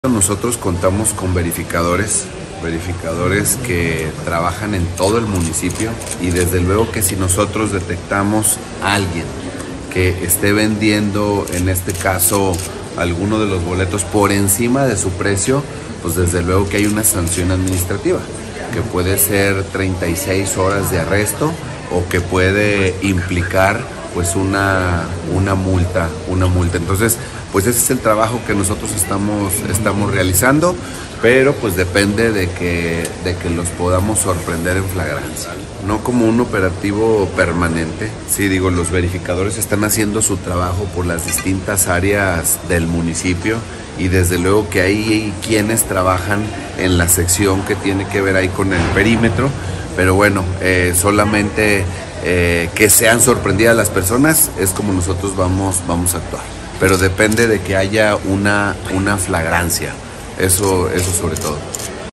Revendedores serán detenidos durante 36 horas: Alcalde Leonardo Montañez
Aguascalientes, Ags. 28 febrero 2023.-  Los revendedores que sean sorprendidos ofreciendo por encima de su precio boletos del Palenque de la Feria o de cualquier otro espectáculo serán detenidos y arrestados durante al menos 36 horas, dejó en claro el Presidente Municipal de Aguascalientes, Leonardo Montañez Castro.
En entrevista colectiva, expuso que los verificadores de la Dirección de Reglamentos municipales se mantendrán alertas para detectar acciones de reventa, en cuyo caso, se detendrá a los responsables para aplicarles sanciones de hasta 36 horas de detención en el C4.
Reconoció sin embargo que para que ello suceda deberán ser sorprendidos en flagrancia.